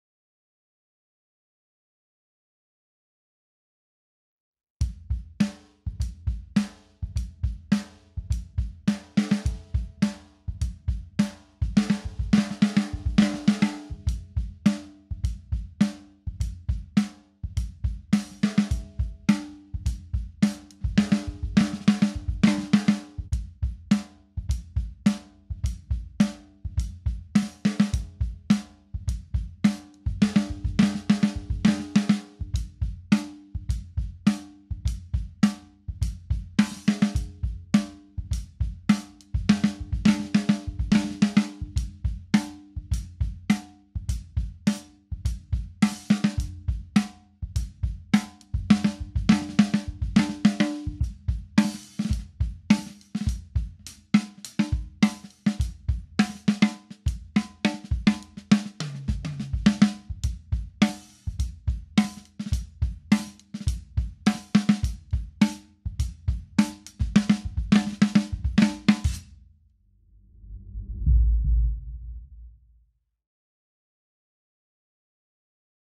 Drops™ demo
This is a live play through demonstrating the tone-shaping effect of Drops™ by Tandem Drums. In this video, different Drops™ are added to the snare drum, one by one, so that you can hear a seamless comparison across different weights.

Drops use gravity to create an active gate effect on the drums, shortening decay without choking the drum's natural tone. Drops are designed by the creator of MONO using recycled materials. They're used by Grammy award-winning drummers, and have won several awards for design.

To learn more about Drops visit tandemdrums.com